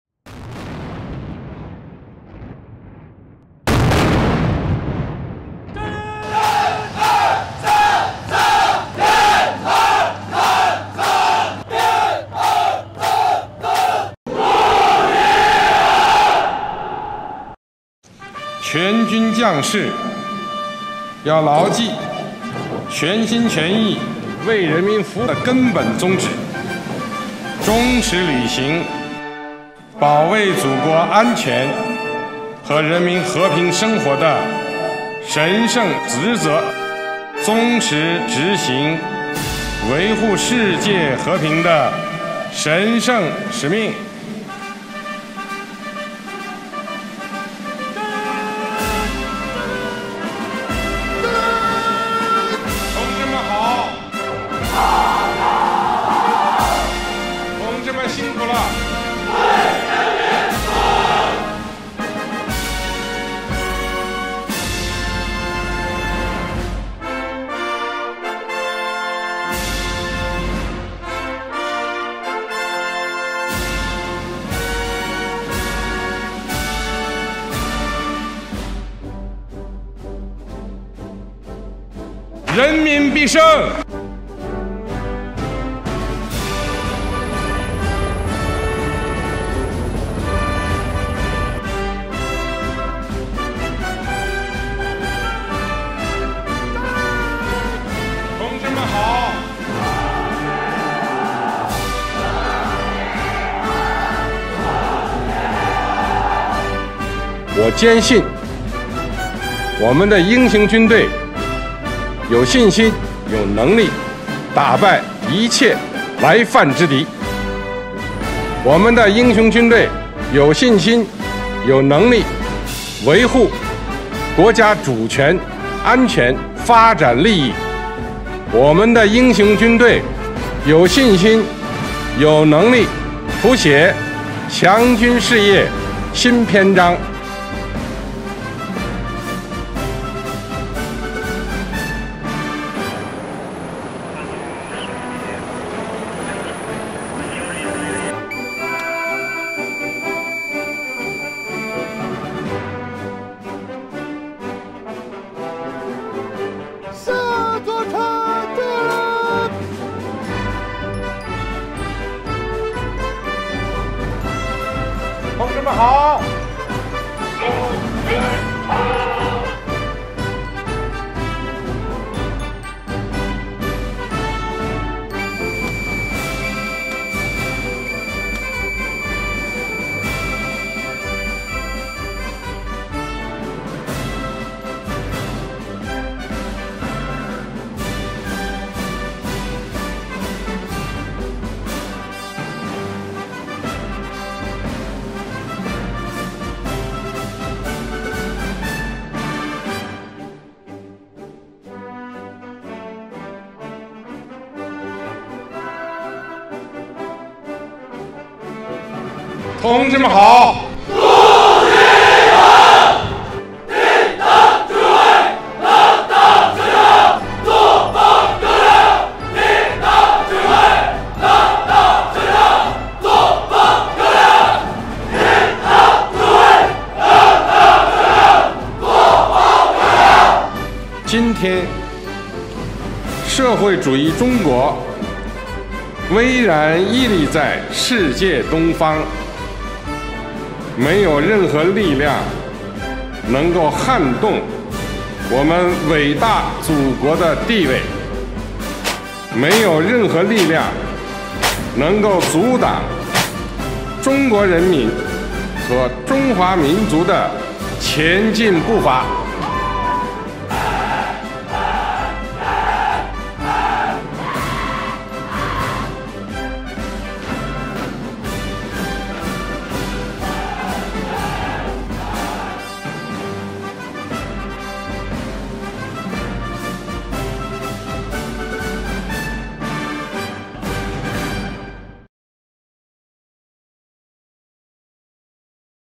China's Army Day: Review of the past four military parades
August 1 marks the 93rd anniversary of the founding of the Chinese People's Liberation Army (PLA). This video reviews the country's last four military parades.